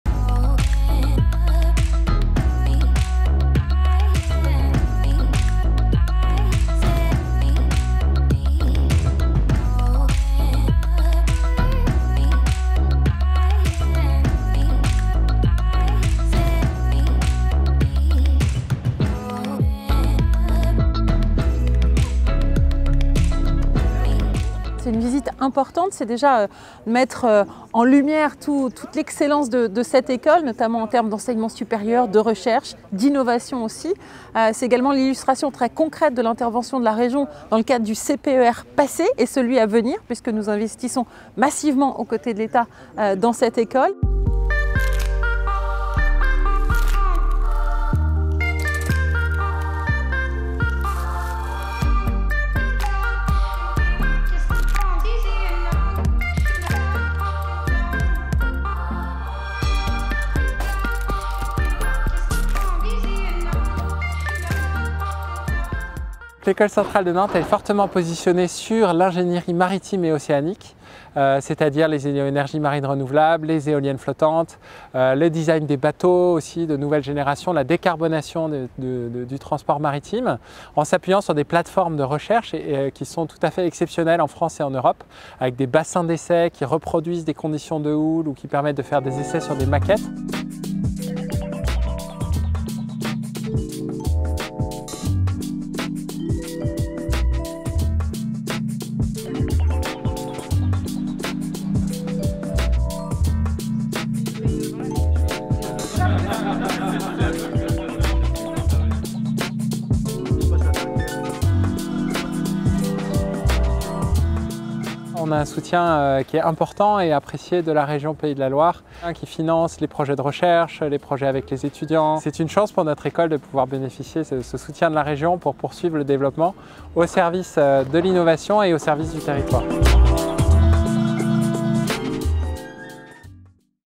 Formation : Christelle Morançais visite l'École Centrale de Nantes (44)
Christelle Morançais, présidente de la Région des Pays de la Loire, était présente le 8 février 2022 à l'Ecole Centrale de Nantes. Elle a été accueillie par son directeur, Jean-Baptiste Avrillier, pour échanger sur plusieurs thématiques de la recherche-innovation, et notamment sur la recherche liée au développement de la filière hydrogène soutenue par la Région. Elle a également rencontré plusieurs start-ups issues de l’incubateur de Centrale Nantes et de la pépinière d’entreprises.